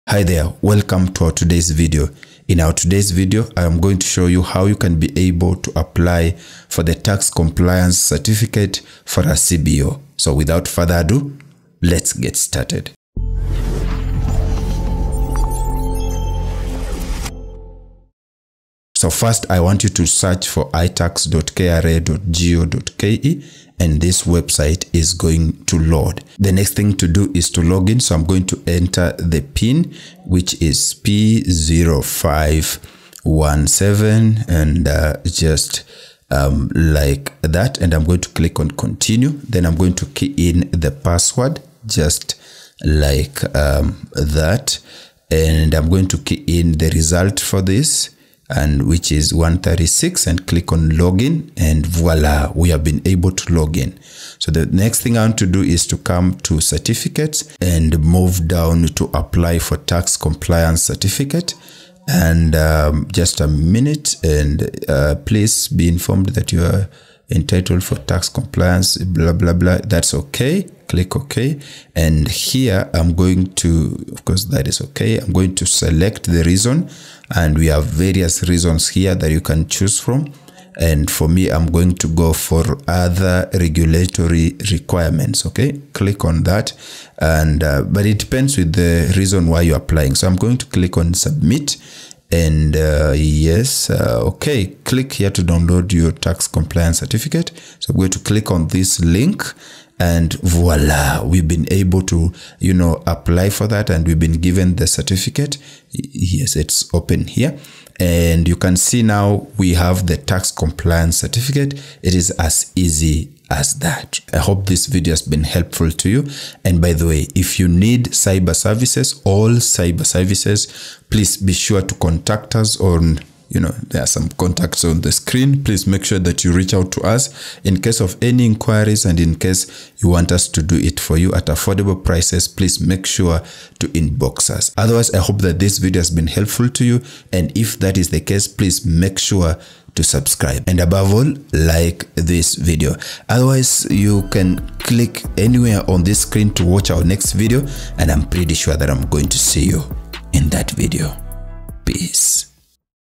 HOW TO APPLY FOR KRA TAX COMPLIANCE CERTIFICATE (FOR INDIVIDUALS, SELF HELP GROUPS AND CBOs)
(Recommended video here)

✪ Ero Productions is free, and will always be.